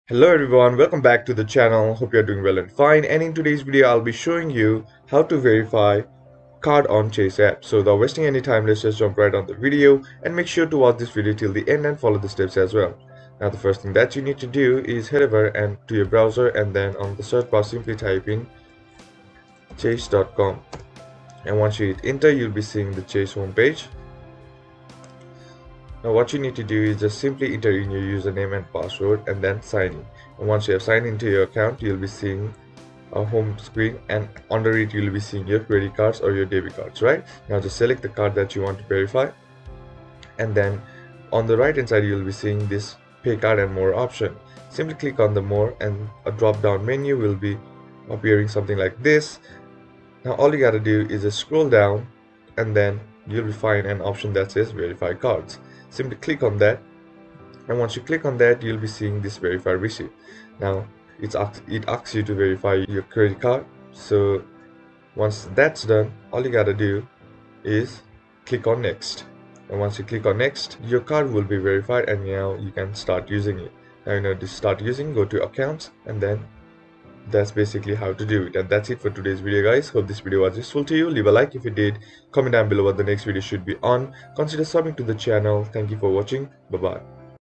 How to Verify Card on the Chase App !! Verify Chase Debit Card Online - 2024 !!
Want to know How To  Verify Card on the Chase App but don't know how?
This quick video will guide you How To Verify Card On Chase App

So, in this few simple steps you can successfully  Verify Card on the Chase App

Timestamps :
00:00 Intro
00:15 Open browser
00:37 Select the card
00:53 Click on verify cards